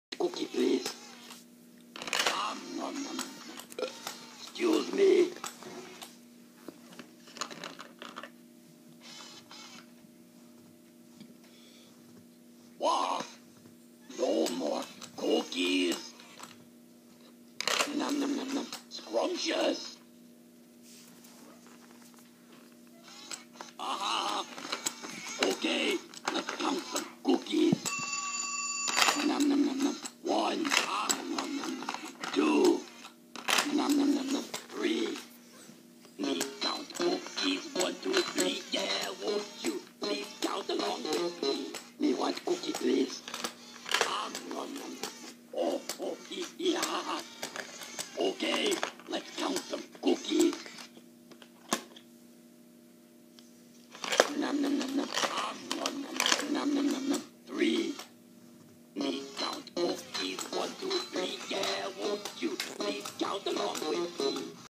Cookie Monster Eating Chips Ahoy Cookie
Monster Was Eating Chewy Cookie